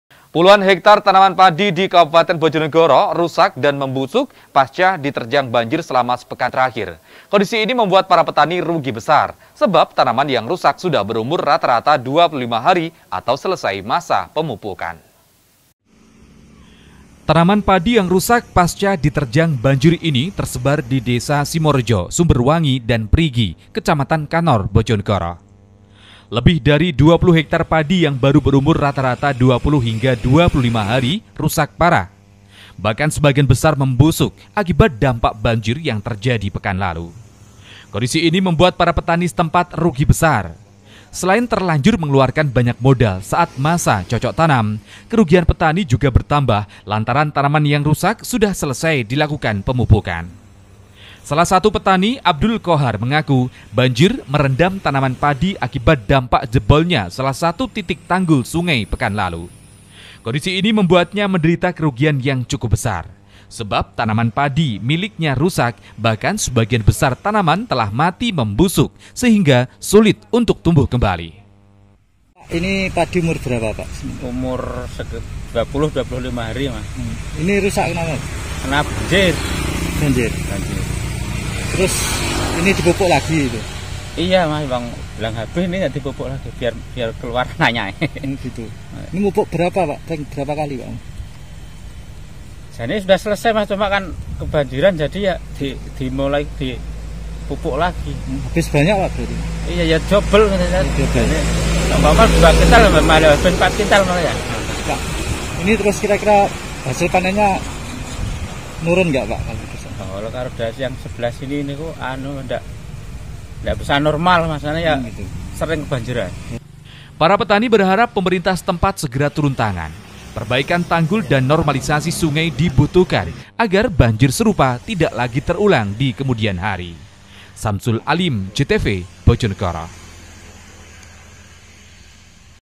BOJONEGORO - Terdampak Banjir, Puluhan Hektar Tanaman Padi Rusak
Puluhan hektar tanaman padi di kabupaten bojonegoro rusak dan membusuk paska diterjang banjir selama sepekan terakhir. Kondisi ini membuat para petani rugi besar/. Sebab tanaman yang rusak sudah berumur rata-rata 25 hari atau selesai masa pemupukan.